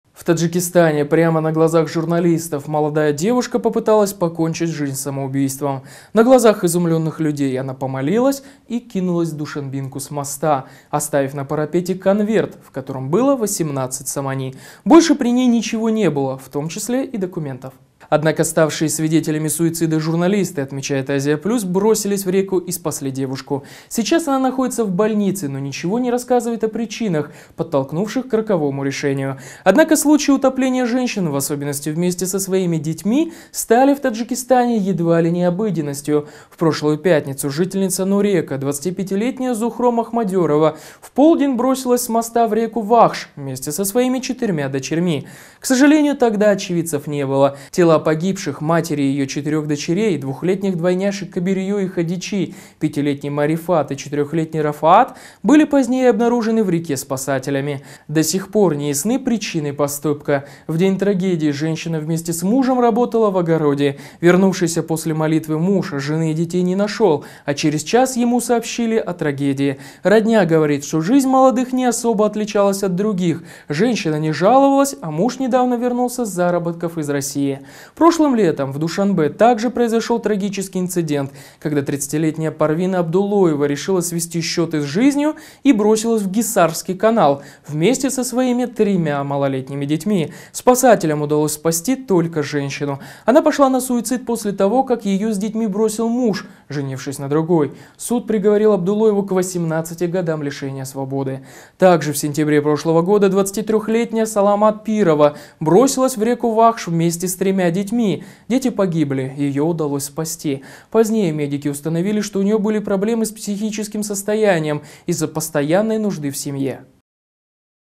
В Таджикистане все больше женщин топится вместе с детьми. Как предотвратить семейные трагедии?
В Таджикистана прямо на глазах журналистов молодая девушка попыталась покончить жизнь самоубийством. На глазах изумленных людей она помолилась и кинулась в Душанбинку с моста, оставив на парапете конверт, в котором было 18 сомони. Больше при ней ничего не было, в том числе и документов.
Однако ставшие свидетелями суицида журналисты, отмечает “Азия Плюс”, бросились в реку и спасли девушку. Сейчас она находится в больнице, но ничего не рассказывает о причинах, подтолкнувших к роковому решению.
Однако случаи утопления женщин в особенности вместе со своими детьми стали в  Таджикистане едва ли не обыденностью.
В прошлую пятницу жительница Нурека, 25-летняя Зухро Махмадёрова в полдень бросилась с моста в реку Вахш вместе со своими четырьмя дочерьми. К сожалению тогда очевидцев не было. Тела погибших – матери и ее четырех дочерей - двухлетних двойняшек Кибериё и Хадичи, пятилетней Марифат и четырехлетней Рафоат были позднее обнаружены в реке  спасателями.
До сих пор неясны причины поступка. В день трагедии женщина вместе с мужем работала в огороде. Вернувшийся после молитвы муж жены и детей не нашел, а через час им сообщили о трагедии. Родня говорит, что жизнь молодых не особо отличалась от других, женщина не жаловалась, а муж недавно вернулся с заработков из России.
Прошлым летом в Душанбе также произошел трагический инцидент, когда 30-летняя Парвина Абдуллоева решила свести счеты с жизнью и бросилась в Гиссарский канал вместе со своими тремя малолетними детьми. Спасателям удалось спасти только женщину. Она пошла на суицид после того, как ее с детьми бросил муж, женившись на другой. Суд приговорил Абдуллоеву к 18 годам лишения свободы.
Также в сентябре прошлого года 23-летняя Саломат Пирова бросилась в реку Вахш вместе с тремя детьми. Дети погибли, ее удалось спасти. Позднее медики установили, что у нее были проблемы с психическим состоянием из-за постоянной нужды в семье.